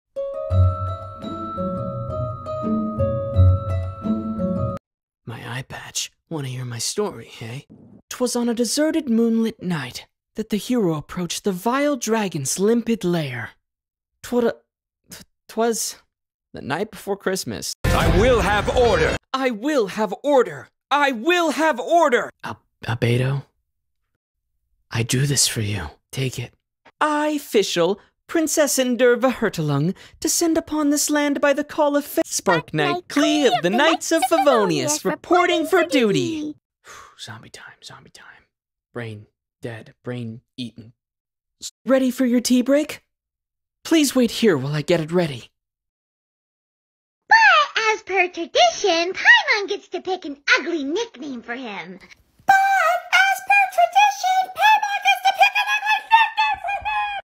Aether voices other Genshin Impact character (animatic)
How am I able to make videos under one minute is a mystery to me

Follow me UwU